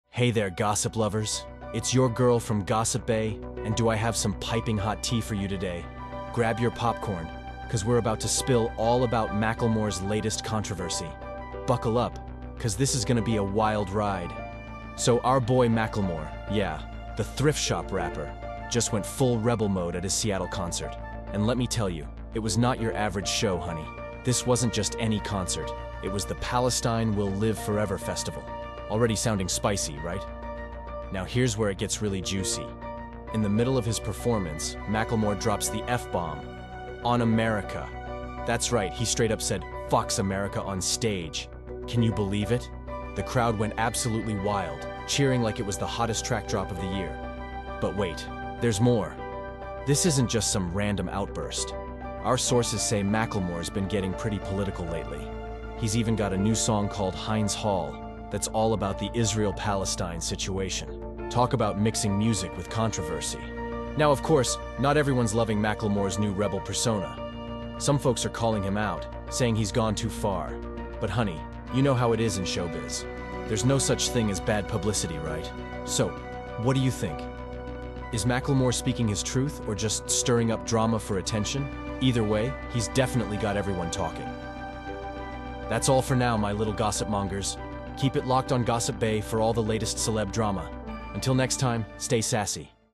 Macklemore Chanting 'F*** America' Onstage at Seattle Concert
In this explosive video, we delve into Macklemore's passionate 'F America' rant during his recent show in Seattle. The hip-hop artist didn't hold back, expressing his thoughts on current social and political issues that resonate deeply with fans and critics alike. Join us as we break down the key moments from the performance, explore the context behind his comments, and discuss the impact of his words in today's climate. Don't miss out on this powerful commentary from one of hip-hop's most vocal artists!